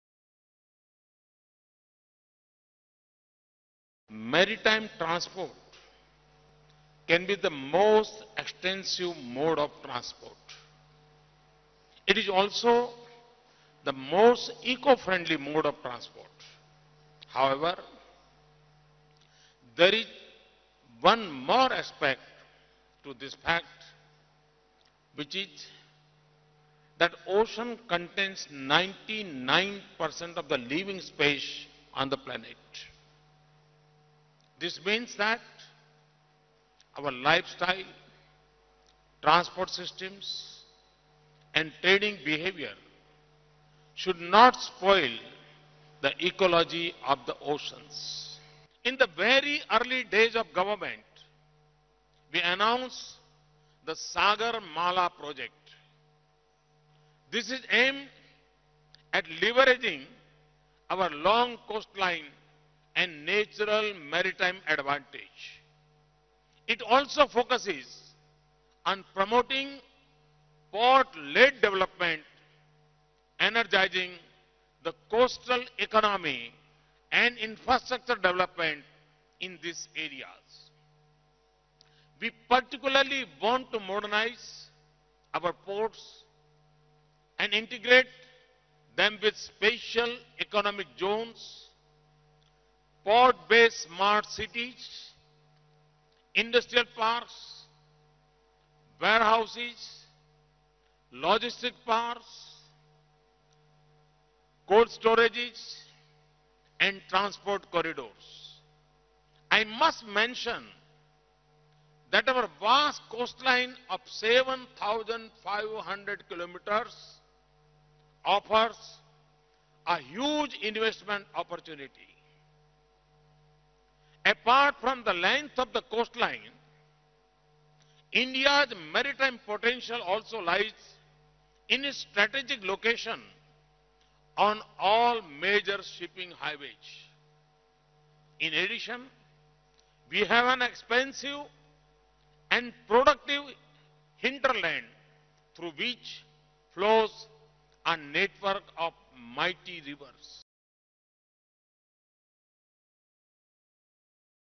Coastal communities can become engine of growth: PM Modi (Part 1 )
Mumbai, Apr 14 (ANI): Speaking at the inauguration of Maritime India Summit 2016 in Mumbai, Prime Minister Narendra Modi on Thursday said maritime transport could be the most extensive mode of transport as it is one of the most eco-friendly modes of transport. PM Modi added that in the early days of his government, they had announced a ‘Sagar Mala’ project, energising coastal economy and boosting infrastructure. The PM added that he wanted to modernise ports and integrate them with SEZs, Port-based smart cities, industrial parks, warehouses, logistics parks, cold storages and transport corridors. Prime Minister Modi said his vision is to increase port capacity from 1, 400 million tonnes to 3,000 million tonnes by 2025. PM Modi on a concluding note said that investing in maritime sector is investing in the planet’s future.